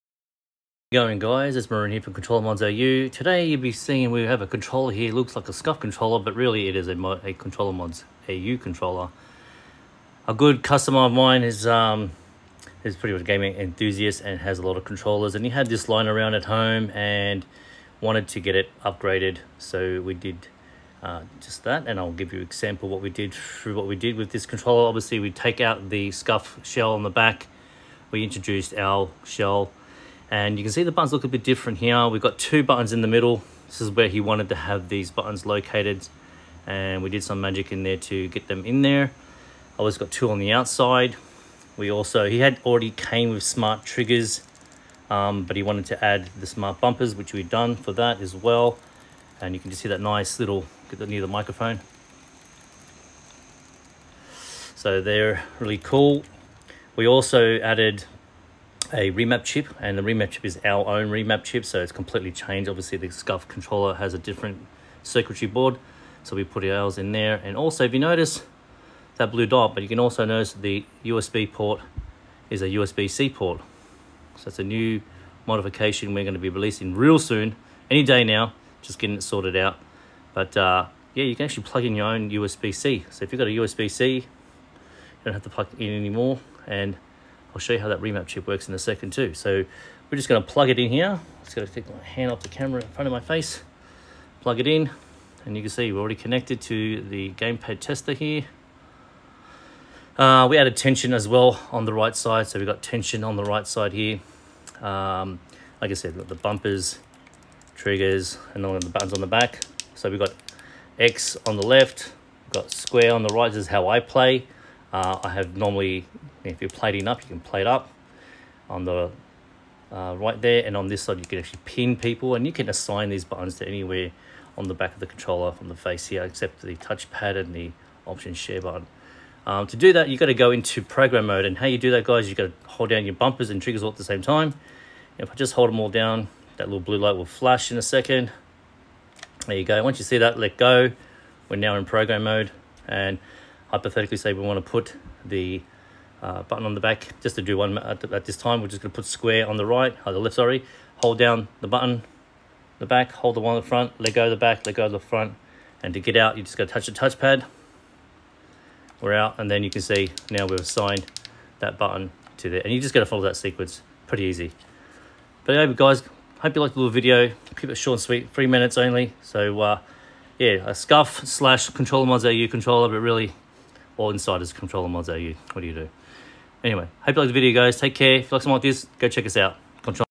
Scuf Controller now a ControllerModzAU controller.
Pretty cool turning this Scuf that had issues into a ControllermodzAU controller.

If you have an old controller we can bring it back to life.

Peace out ✌🏻